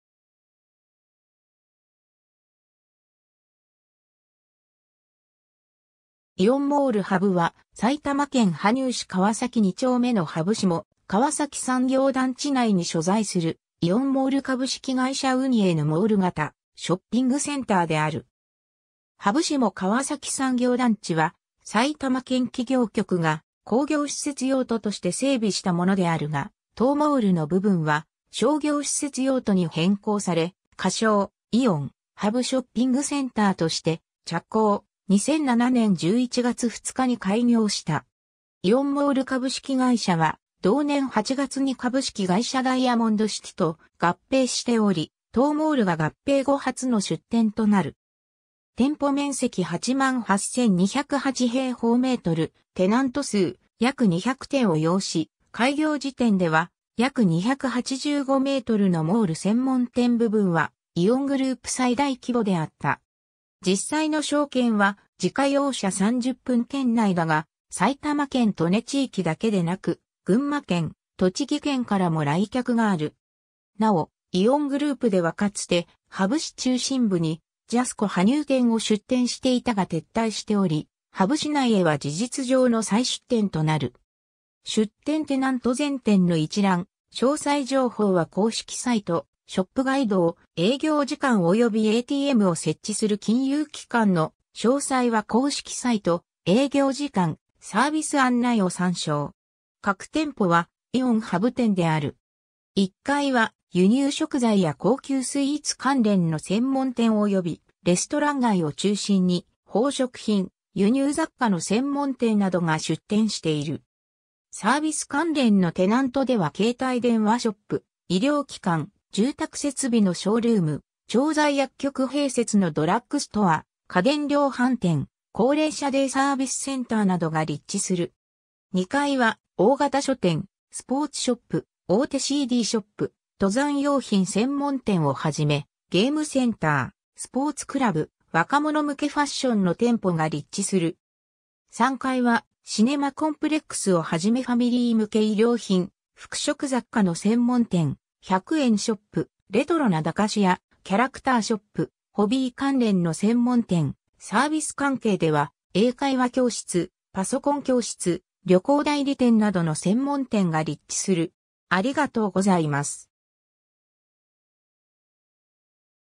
イオンモール羽生
イオンモール羽生（イオンモールはにゅう）は、埼玉県羽生市川崎二丁目の羽生下川崎産業団地内（国道122号、国道125号行田バイパス沿い）に所在するイオンモール株式会社運営のモール型ショッピングセンターである。
 羽生下川崎産業団地は埼玉県企業局が工業施設用途として整備したものであるが、当モールの部分は商業施設用途に変更され、仮称「イオン羽生ショッピングセンター」として着工、2007年（平成19年）11月2日に開業した。
 イオンモール株式会社は同年8月に株式会社ダイヤモンドシティと合併しており、当モールが合併後初の出店となる。
 店舗面積88,208m²・テナント数約200店を擁し、開業時点では約285mのモール専門店部分はイオングループ最大規模であった。
実際の商圏は自家用車30分圏内だが、埼玉県利根地域だけでなく、群馬県、栃木県からも来客がある。
 なお、イオングループではかつて羽生市中心部にジャスコ羽生店を出店していたが撤退しており、羽生市内へは事実上の再出店となる。
 出店テナント全店の一覧・詳細情報は公式サイト「ショップガイド」を、営業時間およびATMを設置する金融機関の詳細は公式サイト「営業時間・サービス案内」を参照。
 核店舗はイオン（旧・ジャスコ）羽生店である。
1階は輸入食材や高級スイーツ関連の専門店およびレストラン街を中心に、宝飾品、輸入雑貨の専門店などが出店している。
サービス関連のテナントでは携帯電話ショップ、医療機関、住宅設備のショールーム、調剤薬局併設のドラッグストア、家電量販店、高齢者デイサービスセンターなどが立地する。
 2階は大型書店、スポーツショップ、大手CDショップ、登山用品専門店を始め、ゲームセンター、スポーツクラブ、若者向けファッションの店舗が立地する。
 3階はシネマコンプレックスをはじめファミリー向け衣料品、服飾雑貨の専門店、100円ショップ、レトロな駄菓子屋、キャラクターショップ、ホビー関連の専門店、サービス関係では英会話教室、パソコン教室、旅行代理店などの専門店が立地する。